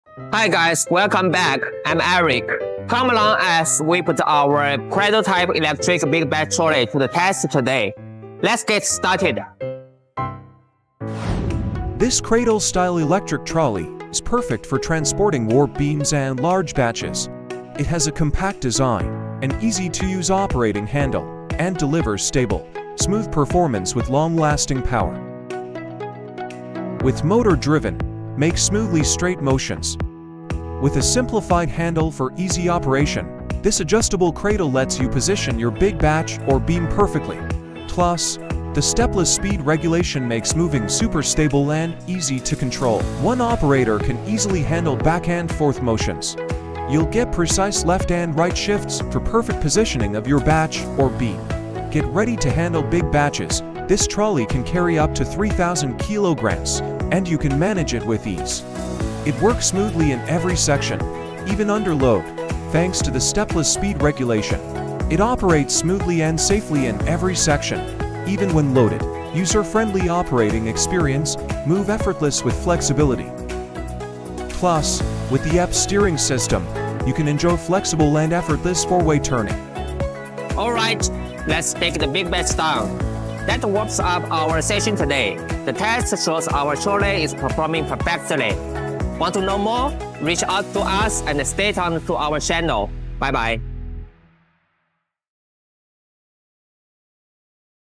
Electric Warp Beam Carrier ST-MBT-01II Test: Easy Operation, Time-Saving&Effortless!
SUNTECH's Electric Warp Beam Carrier adopts a Cradle design, which can not only easily transport beams, but also flexibly handle batches, greatly improving production efficiency. ✅The compact body design allows for flexible operation in a small space while also having a large load capacity. Multiple humanized designs make operation more convenient, and only one person is required to operate it easily. 🧑‍🏭🆗